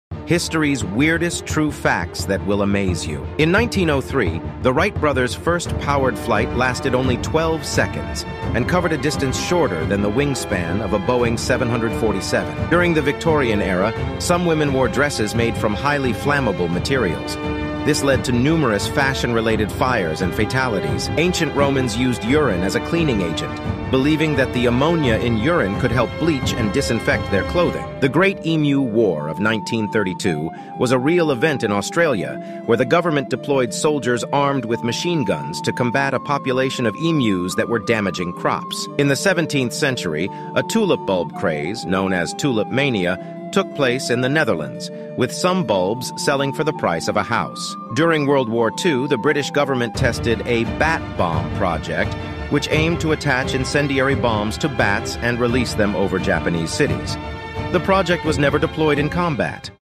History's Weirdest True Facts That Will Amaze You 🤯
Step into the bizarre and unbelievable world of history as we unveil 'History's Weirdest True Facts That Will Amaze You.' Prepare to be astonished as we delve into the oddest, most mind-boggling tales from the annals of the past.
In 1903, the Wright brothers made history with their first powered flight, which lasted just 12 seconds and covered a distance shorter than the wingspan of a Boeing 747! 🛩️
Venture back to the Victorian era, where fashion could be deadly. Some women wore dresses made from highly flammable materials, leading to fashion-related fires and tragic fatalities. 🔥
Ancient Romans, with their unique sense of cleanliness, used urine as a cleaning agent, believing it had the power to bleach and disinfect their clothing. 🧼
Ever heard of the Great Emu War of 1932? It was a real event in Australia where the government deployed soldiers armed with machine guns to combat a population of emus that were wreaking havoc on crops! 🦙💥
Travel back to the 17th century and witness the 'Tulip Mania' in the Netherlands, where tulip bulb prices soared to the level of a house! 🌷💰
And during World War II, the British government embarked on an extraordinary project – the 'bat bomb.' The plan? Attach incendiary bombs to bats and release them over Japanese cities. Discover why this bat-tastic scheme never saw combat. 🦇💣
Prepare to have your mind blown by these truly weird and wonderful historical facts. 📽️✨